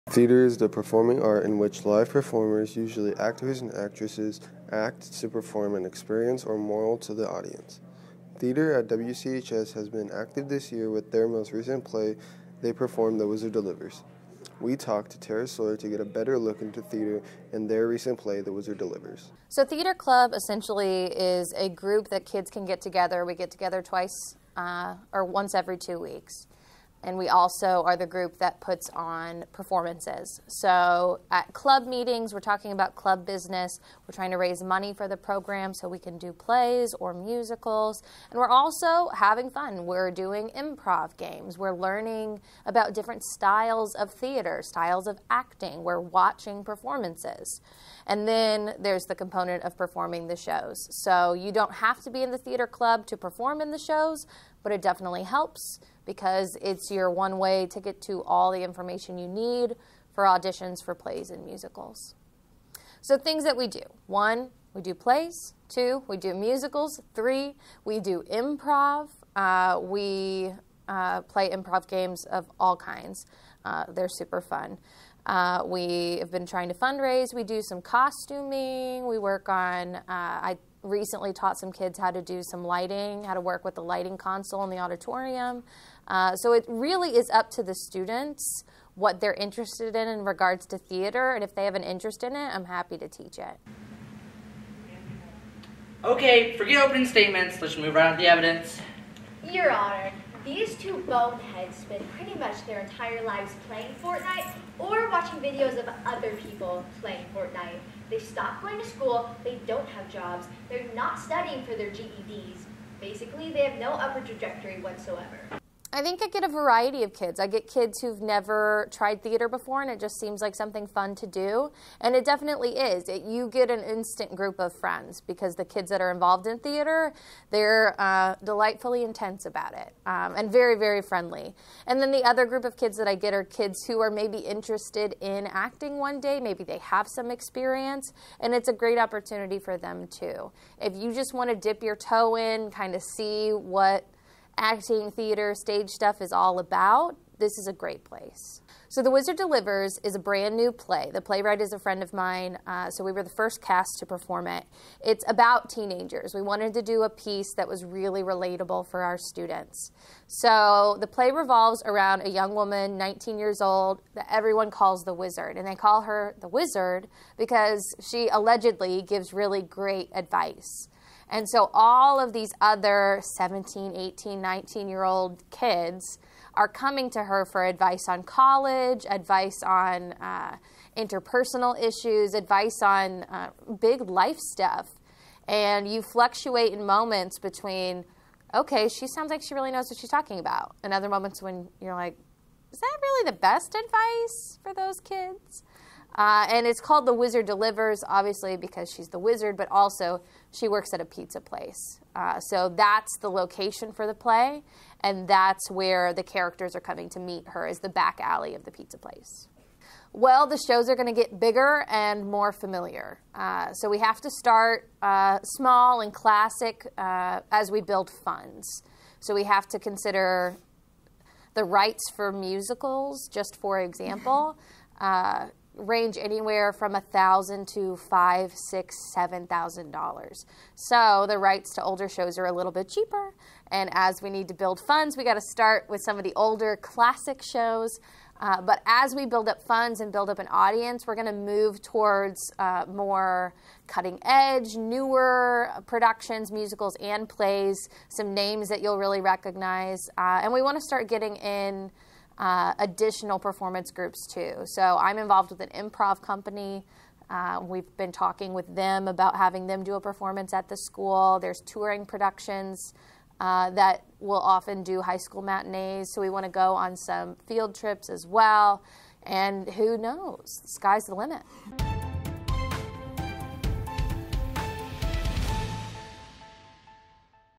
Fall Play: The Wizard Delivers
WHEN catches up with Whiteland Theater director Tara Sorg to talk about this past fall's play, "The Wizard Delivers" and learn more about the Theater Department as a whole.

Credits: Jordan Wrightman